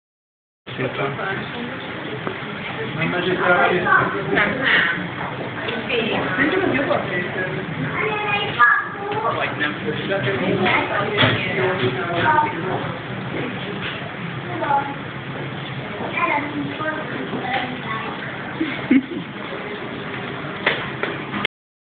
Lunchtime in the Budapest jungle
A lizard eating leaves in the Budapest Zoo.